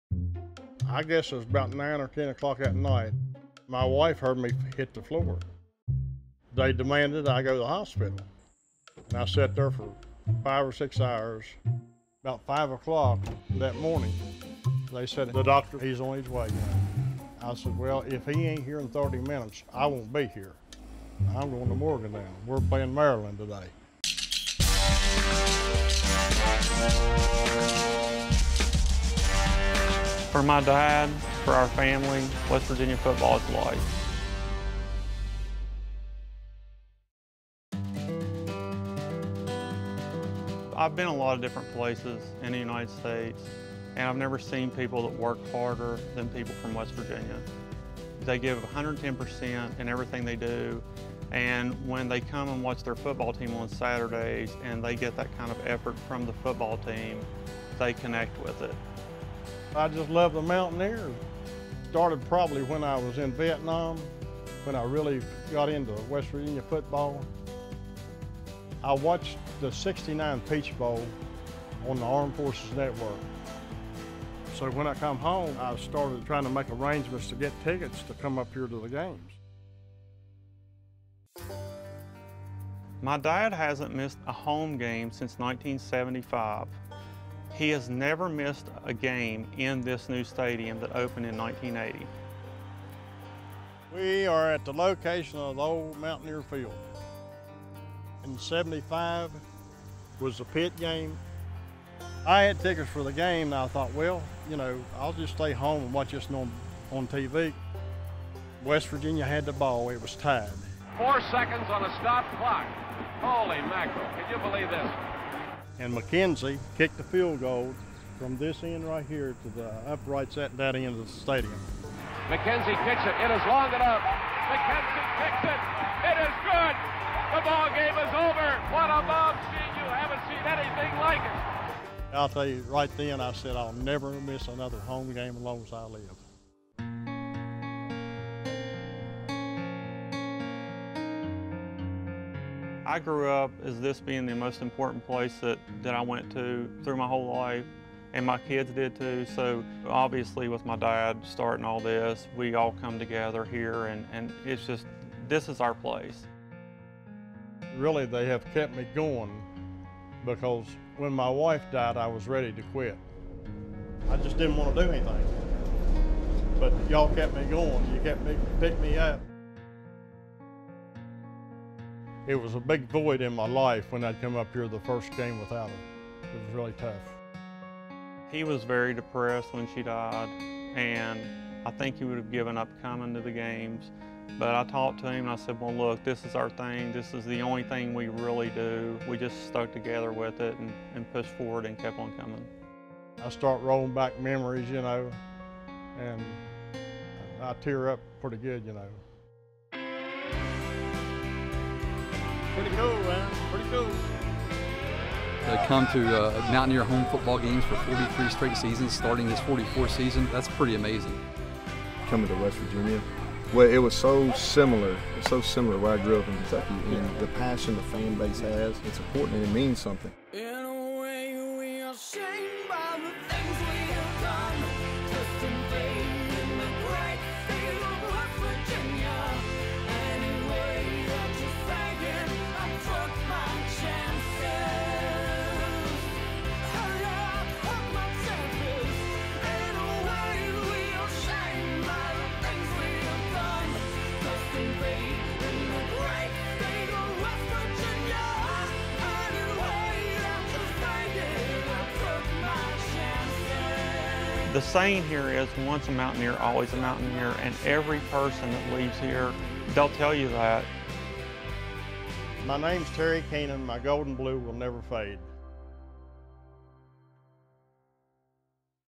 College Colors Day WVU Fan Story
In 1969, the West Virginia Mountaineers played the South Carolina Gamecocks in the Peach Bowl. Half way around the world, Terry Keenan was serving in Vietnam and was able to watch the game on the Armed Forces Network. 50 years later, Terry is one of WVU's most passionate fans, attending every home game since 1975. This is the story of football, family and the love for the state of West Virginia.